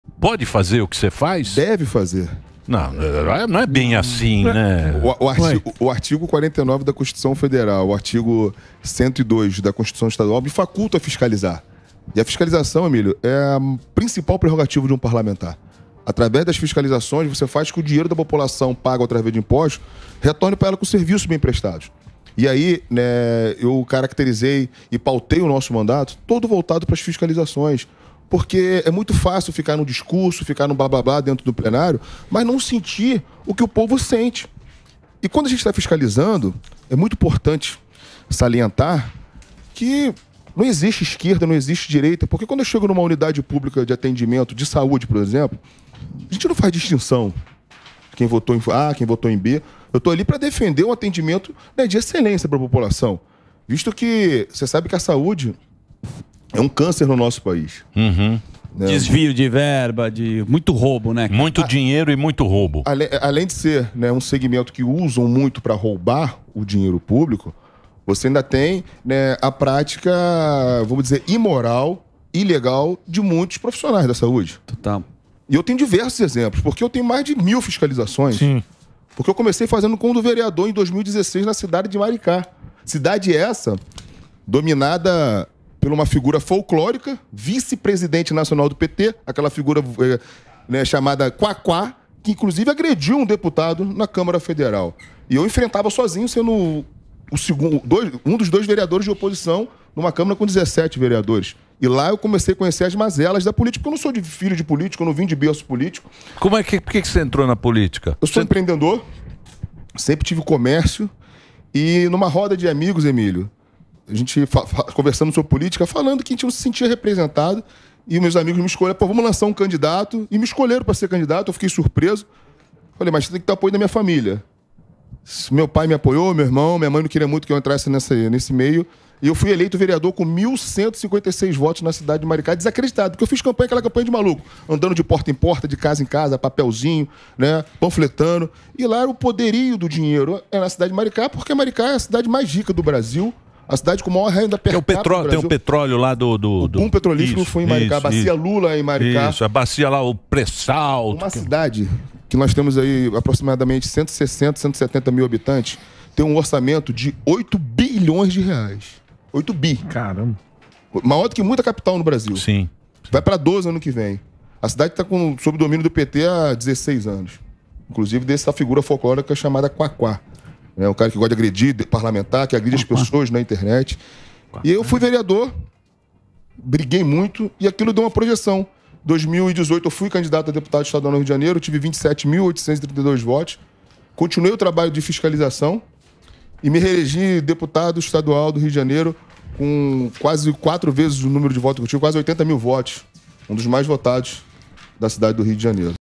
DEPUTADO FILIPPE POUBEL FALA SOBRE FISCALIZAÇÃO DE HOSPITAIS: PODE ISSO, ARNALDO?
O deputado estadual Filippe Poubel (PL-RJ) comenta sobre seus vídeos que bombaram nas redes sociais, nos quais ele faz uma espécie de tour visitando hospitais do SUS para fiscalizar a qualidade dos serviços públicos nas unidades de saúde. Mas, afinal, pode isso?